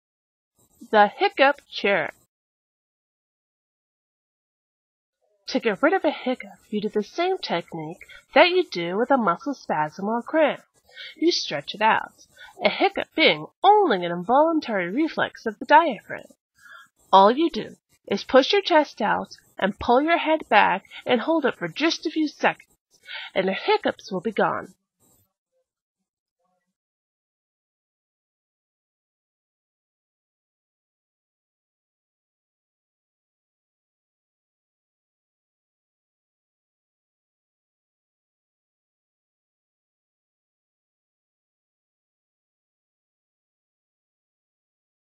THE HICCUP CURE
this is how to cure the common hiccups in the style of infomercials.